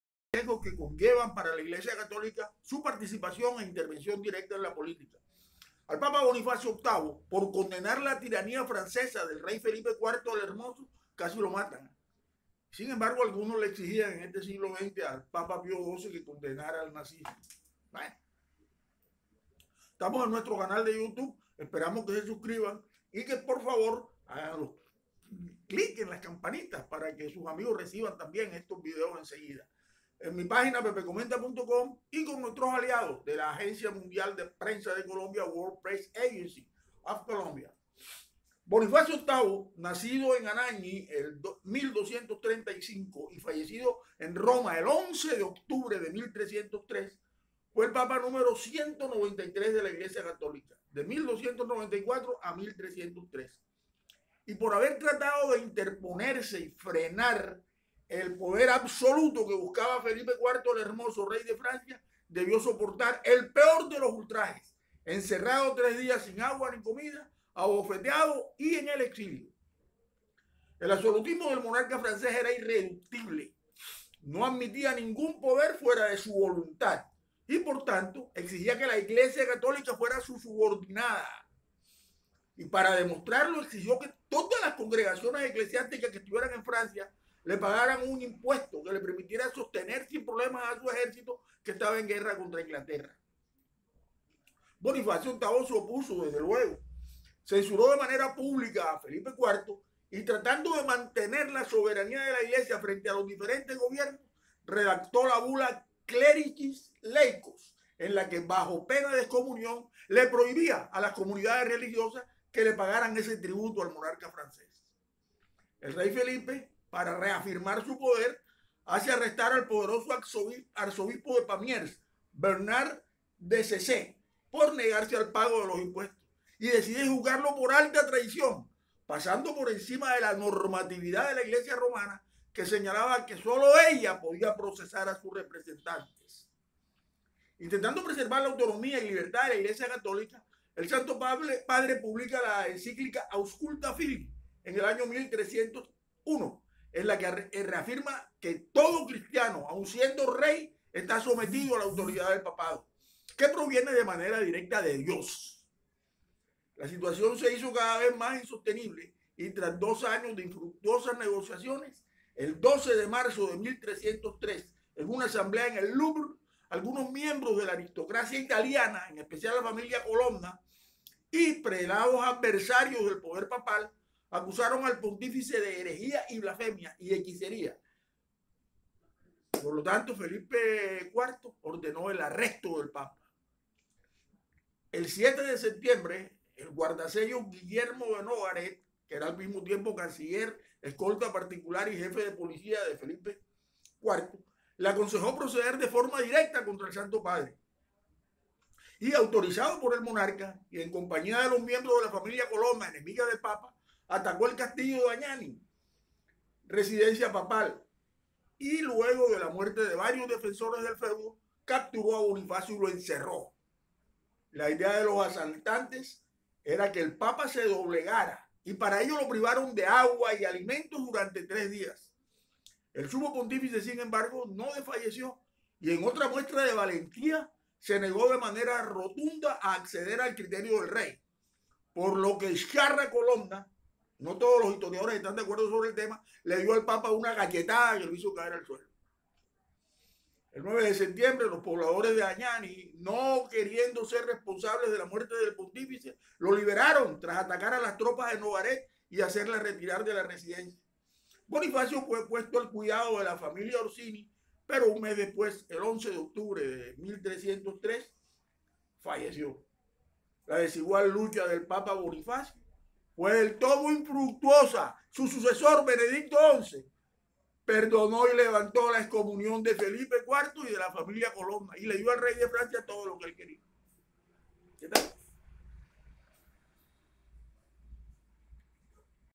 11 de octubre de 1303 fallece el Papa Bonifacio VIII. Condenó tiranía francesa y casi muere por eso
Bonifacio VIII  nacido en Anagni 1235  y fallecido en Roma, 11 de octubre de 1303, fue el  Papa #103 de la Iglesia Católica, de 1294 a 1303, y por haber tratado de interponerse y frenar el poder absoluto que buscaba Felipe IV El Hermoso, Rey de Francia, debió soportar el peor de los ultrajes, encerrado tres días sin agua y comida, abofeteado y en el exilio.
El absolutismo del monarca francés era irreductible: no admitía ningún poder fuera de su voluntad y por tanto, exigía que la Iglesia Católica fuese su subordinada. Para demostrarlo, exigió que todos las congregaciones eclesiásticas de su reino le pagaran un tributo que le permitiera sostener sin problemas su ejército que guerreaba contra Inglaterra.